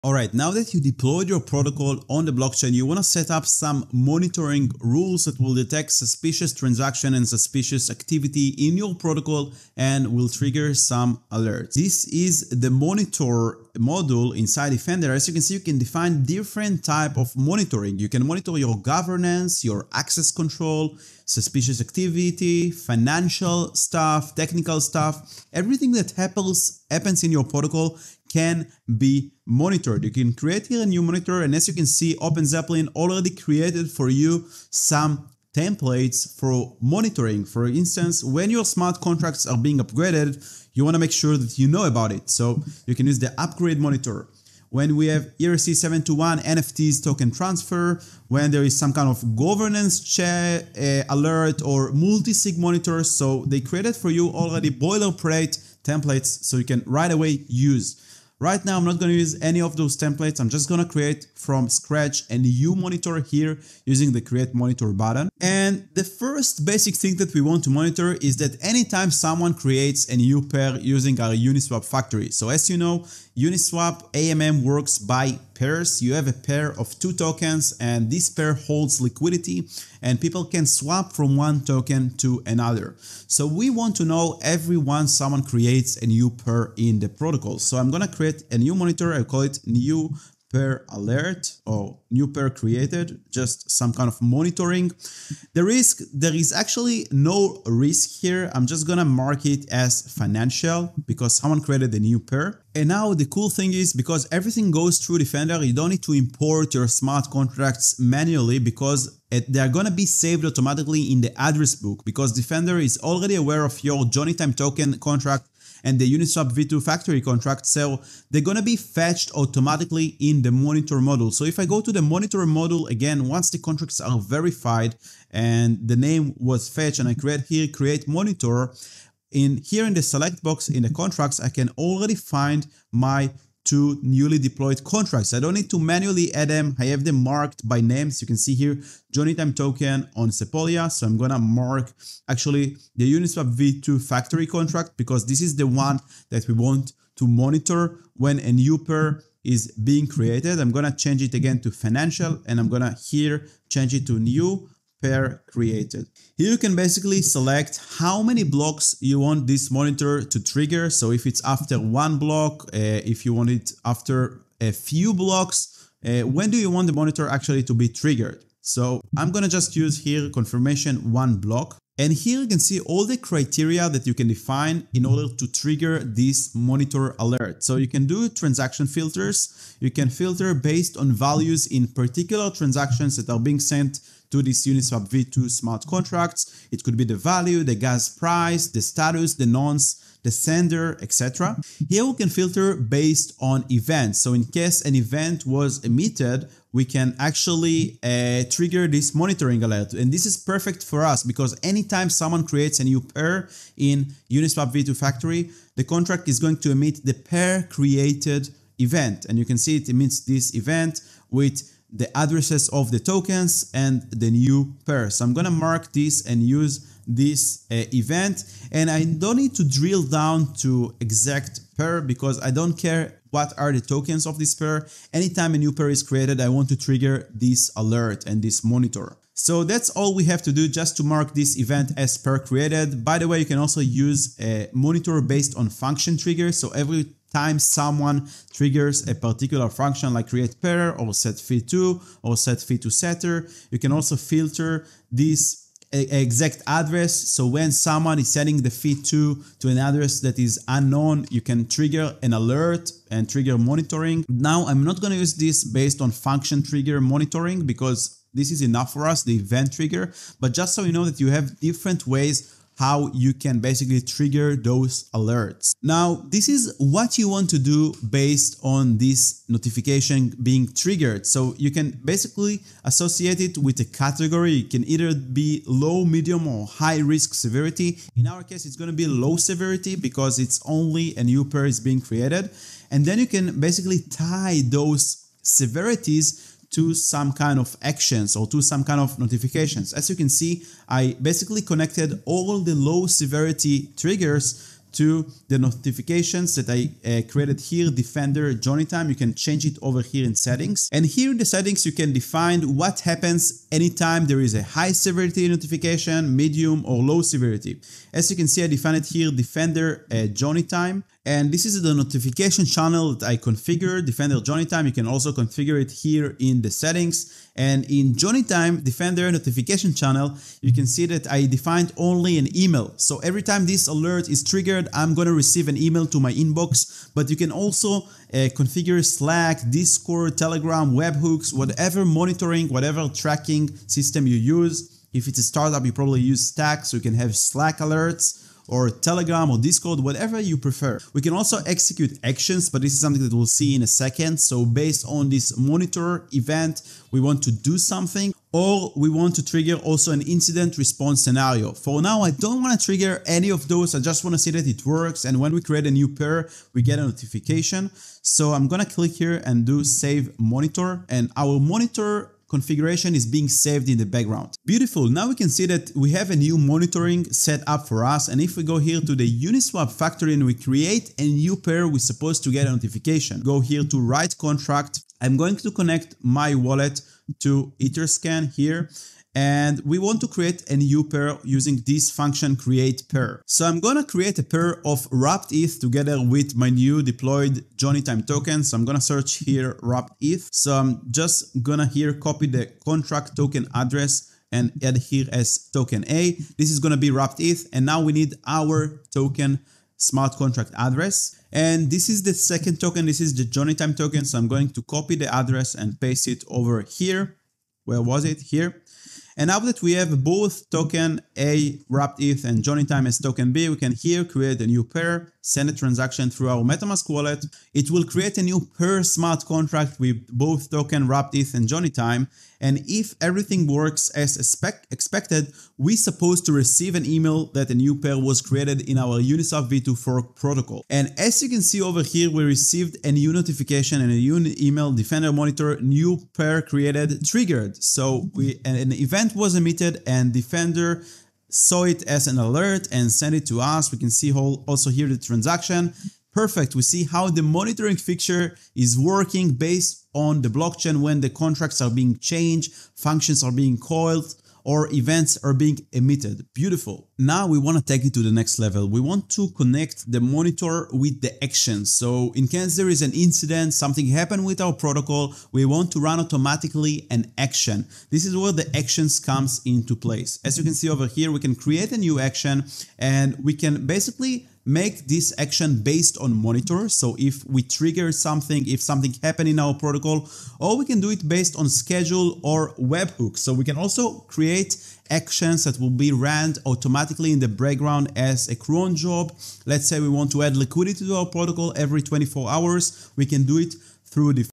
Monitoring Smart Contracts Activities Using OpenZeppelin Defender
In this video, we will guide you through the powerful features of Defender's monitoring module, offering a deep understanding of monitoring types, creation processes, and practical applications.

🛠️ What We Will Cover:
- An insightful overview of OpenZeppelin Defender's robust monitoring capabilities.
- Definition and exploration of various monitoring types
- A step-by-step guide as we walk you through creating monitors from scratch.
- We will demonstrate monitoring Uniswap Factory for new pair creations.
- Detailed configuration walkthrough, where we will cover trigger conditions and set up notification channels.

👨‍💻 Who This Guide Is For:
This tutorial is tailored for blockchain developers, smart contract auditors, and anyone eager to enhance the security of their Web3 projects.

👉 Timestamps:

0:00 OpenZeppelin Boilerplate Monitor Templates
1:00 Creating a New Monitor Rule
4:24 Configuring Notification Channels
6:05 Testing Our Monitor Rule